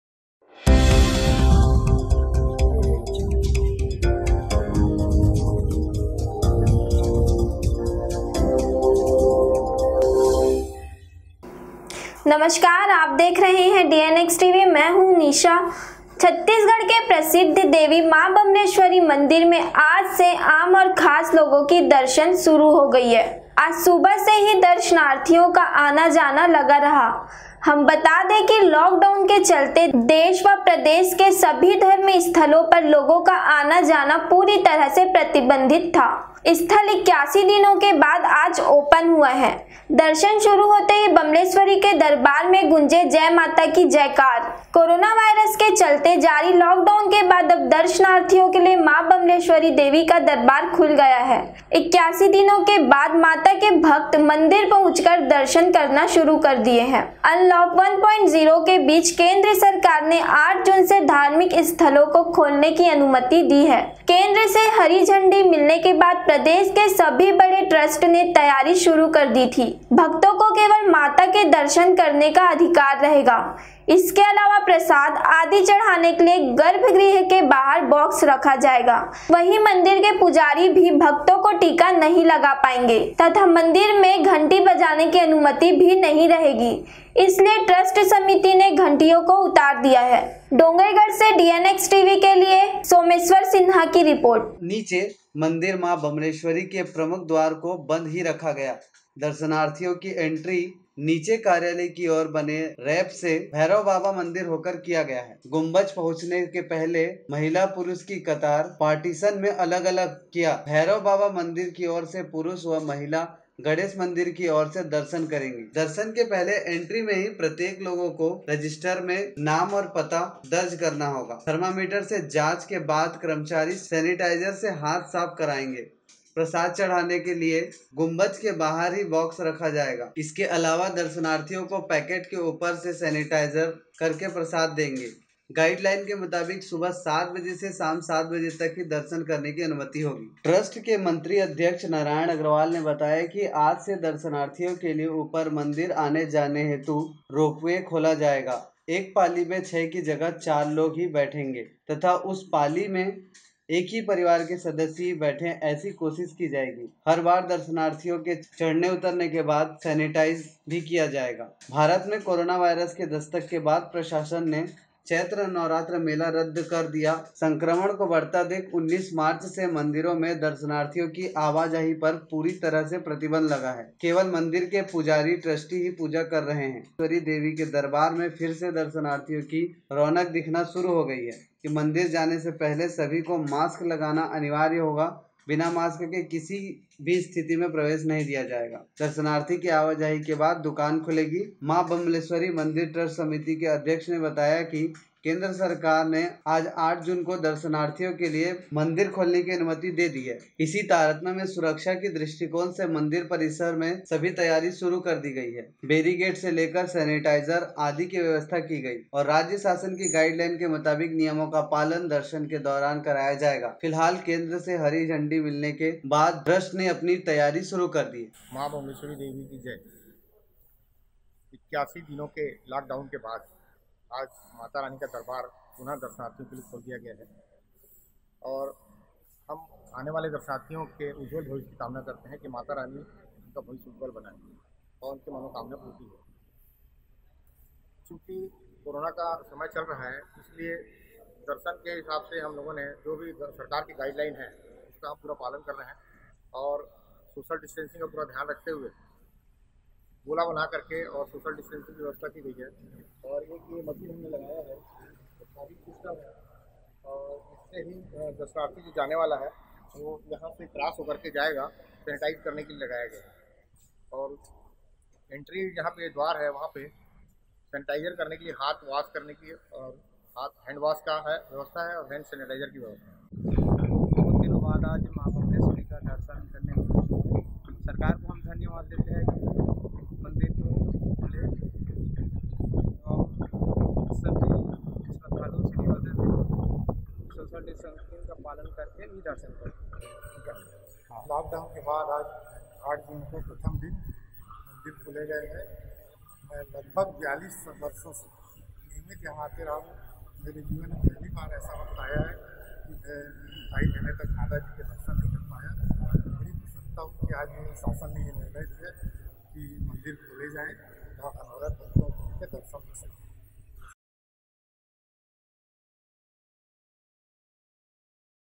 देवी माँ बम्लेश्वरी मन्दिर में आज से दर्शन शुरू | Maa Bamleshwari Temple Chhattisgarh
छत्तीसगढ़ के प्रसिद्ध देव माँ बम्लेश्वरी मन्दिर में आज से आम और खास लोगो की दर्शन सुरु हो गया आज सुबह से ही दर्शनार्थियों का आना जाना लगा रहा हम बता दे की लॉक डाउन के चलते देश व प्रदेश के सभी धर्म स्थलों पर लोगो के आना जाना पूरी तरह से प्रतिबंधित था  स्थल81 दिनों के बाद आज ओपन हुआ है  दर्शन सुरु होते ही  बम्लेश्वरी का दरबार में गूंजे  जय माता की जय कार

कोरोना वायरस के चलते जारी लॉक डाउन के बाद अब दर्शनार्थियों के लिए मां बम्लेश्वरी देवी का दरबार खुल गया  है 81 दिनों के बाद माता के भक्त मंदिर पहुंचकर दर्शन करना सुरु कर दिए है  अनलॉक 1.0 के बीच केंद्र सरकार ने 8 जून से धार्मिक स्थलों को भी खोलने की अनुमति दी है केंद्र से हरी झंडी मिलने के बाद प्रदेश के सभी बड़े ट्रस्ट ने तैयारी शुरू कर दी थी  भक्तों को केवल माता के दर्शन करने का अधिकार रहेगा इसके अलावा प्रसाद आदि चढ़ाने के लिए गर्भगृह  के बाहर बॉक्स रखा जाएगा वहीं मंदिर के पुजारी भी भक्तों को टीका भी नहीं लगाएंगे तथा मंदिर में घंटी बजाने की अनुमति भी नहीं रहेगी इसलिए ट्रस्ट समिति ने घंटियों को उतार लिया है